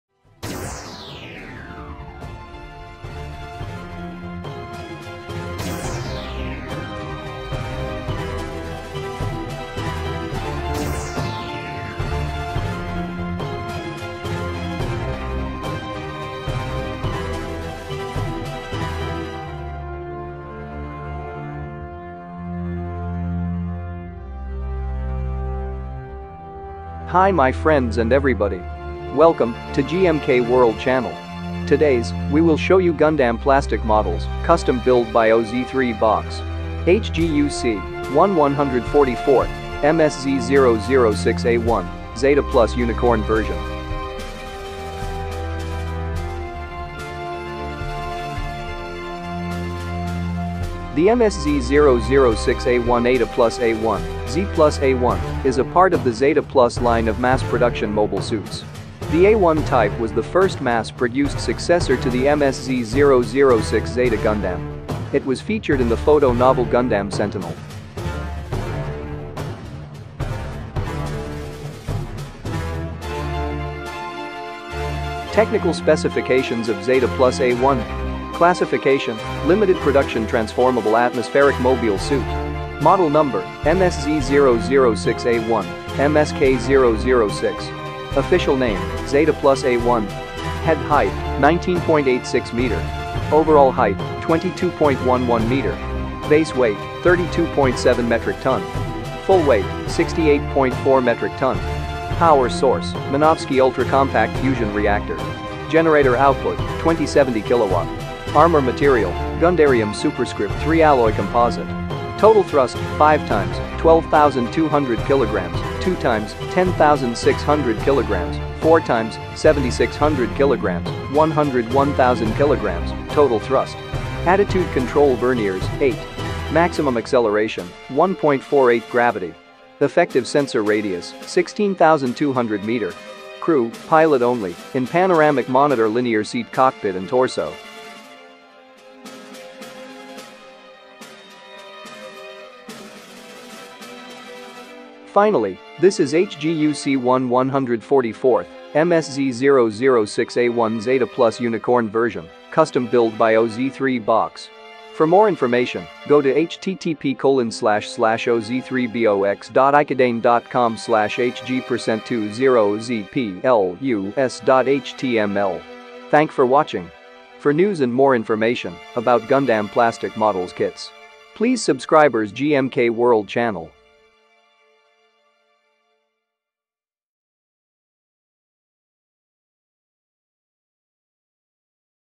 HGUC 1/144 MSZ-006A1 Zeta Plus [Unicorn Ver] - Customized Build(ゼータプラス ユニコーンVer)
Modelled By oz3box

The MSZ-006A1 Ζeta Plus A1 (Z Plus A1) is a part of the Zeta Plus line of mass-production mobile suits. The A1 type was the first mass-produced successor to the MSZ-006 Zeta Gundam. It was featured in the photo-novel Gundam Sentinel.

Technical Specifications of Zeta Plus A1
Classification - Limited Production Transformable Atmospheric Mobile Suit
Model Number - MSZ-006A1, MSK-006
Official Name - Zeta Plus A1
Head Height - 19.86 m
Overall Height - 22.11 m
Base Weight - 32.7 t
Full Weight - 68.4 t
Power Source - Minovsky Ultracompact Fusion Reactor
Generator Output - 2070 kW
Armor Material - Gundarium γ Alloy Composite
Total Thrust - 5 x 12200 kg, 2 x 10600 kg, 4 x 7600 kg, 101000 kg (total thrust)
Attitude Control Verniers - 8
Maximum Acceleration - 1.48 G
Effective Sensor Radius - 16200 m
Crew - Pilot only (in panoramic monitor/linear seat cockpit in torso)